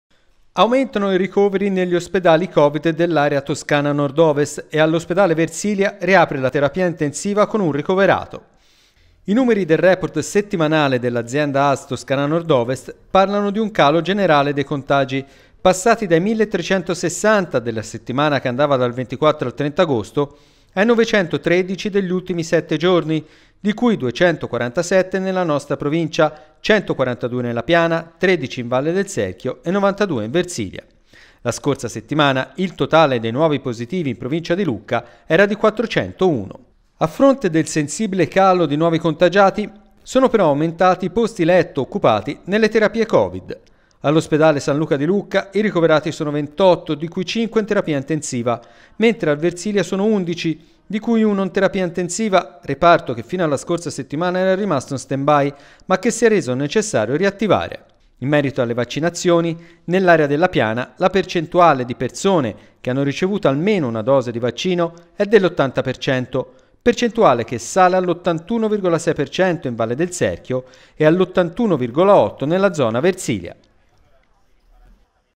Riapre la terapia intensiva Covid al Versilia
Aumentano i ricoveri negli ospedali Covid dell'area Toscana Nord Ovest e all'Ospedale Versilia riapre la terapia intensiva con un ricoverato